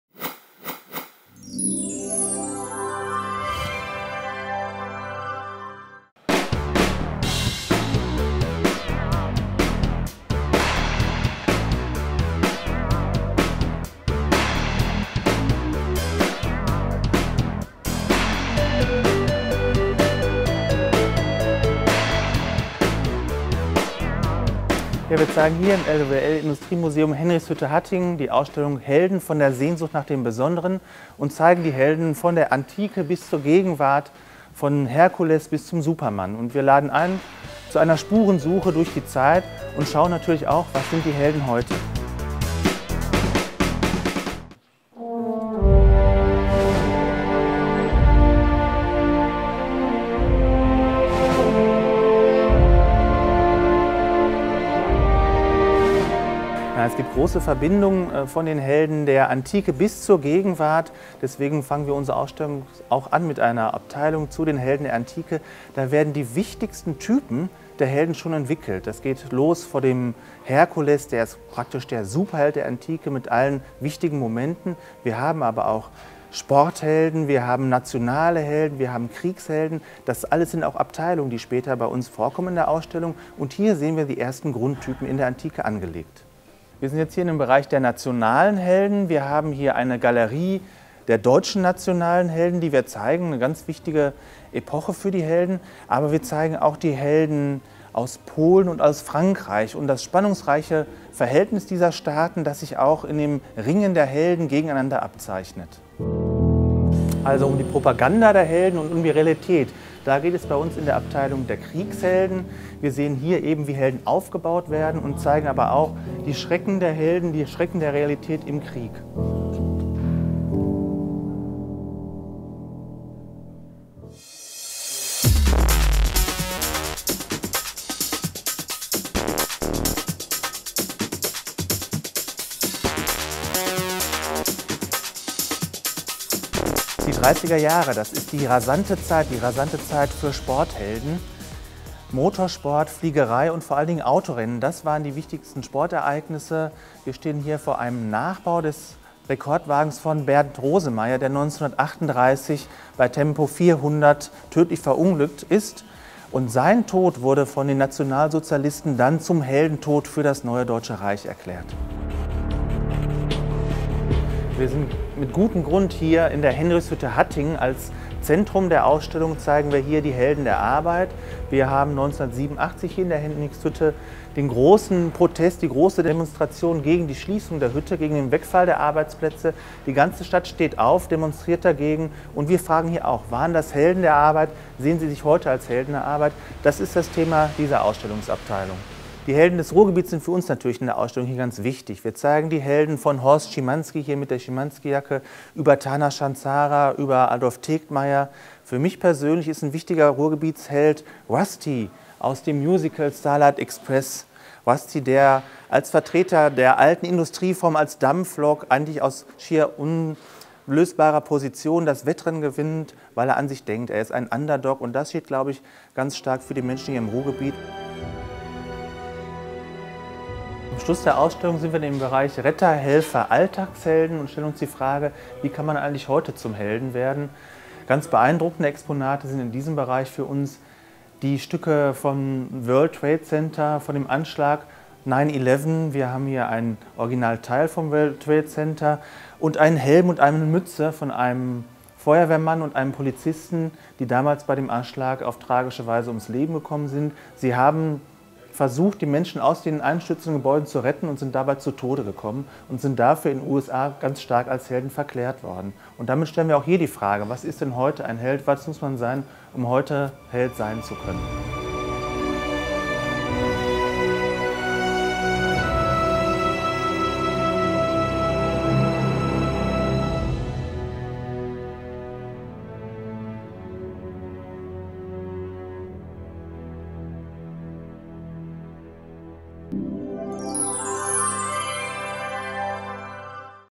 LWL-Ausstellung HELDEN - Von der Sehnsucht nach dem Besonderen, Henrichshütte Hattingen
HELDEN - Von der Sehnsucht nach dem Besonderen

12. März bis 31. Oktober 2010, LWL-Industriemuseum Henrichshütte, Hattingen

Ob Herkules oder Schimanski, Jeanne dArc oder Lara Croft  jede Zeit und jede Kultur hat ihre Heldinnen und Helden. Sie werden gemacht, verehrt, gefeiert und gestürzt. Das zeigt die Ausstellung „HELDEN. Von der Sehnsucht nach dem Besonderen, die der Landschaftsverband Westfalen-Lippe (LWL) in Zusammenarbeit mit der Kulturhauptstadt Europas RUHR.2010 in seinem Industriemuseum Henrichshütte Hattingen präsentiert.

Filmproduktion: dreamJ Videolösungen
Herne, Ruhrgebiet